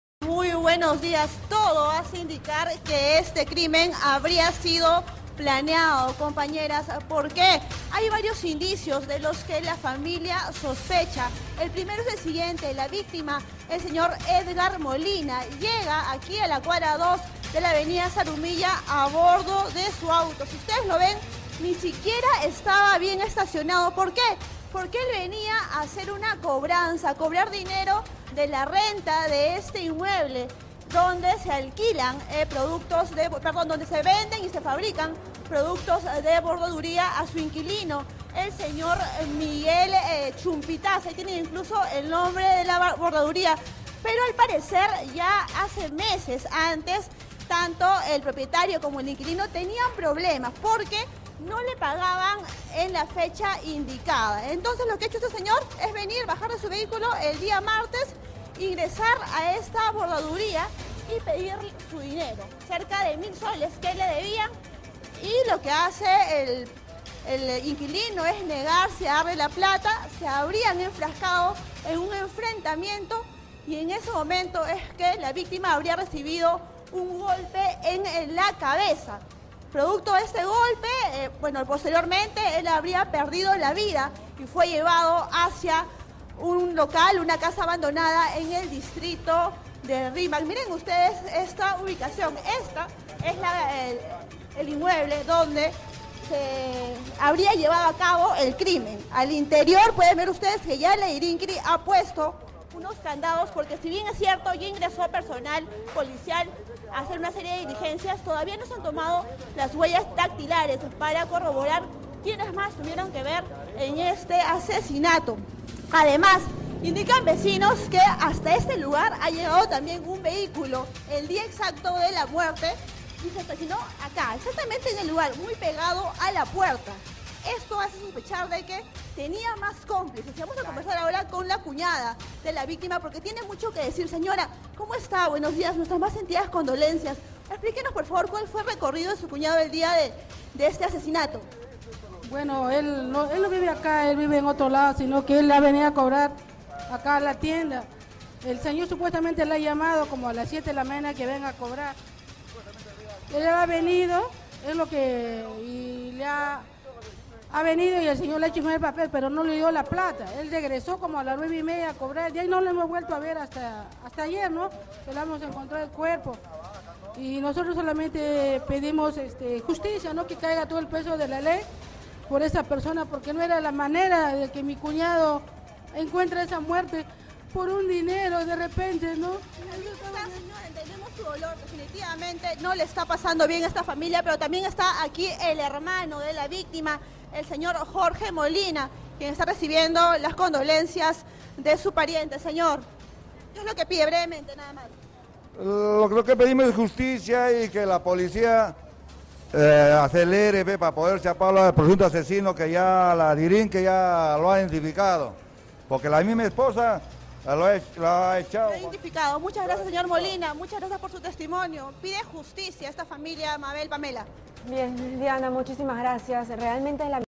Exigen justicia para hombre que fue hallado muerto en casa abandonada del Rímac (1/2)
Autores del crimen lo enterraron en la vivienda abandonada.